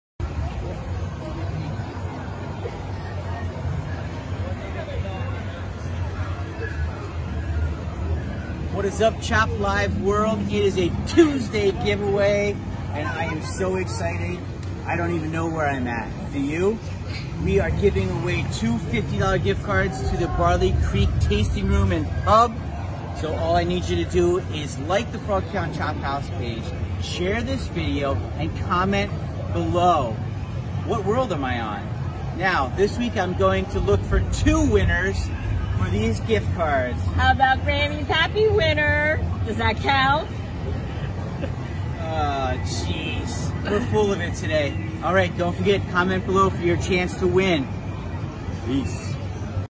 What world is Lyman in for this Tuesday Giveaway??
It’s Tuesday Giveaway day!! Can you guess what world Lyman is in?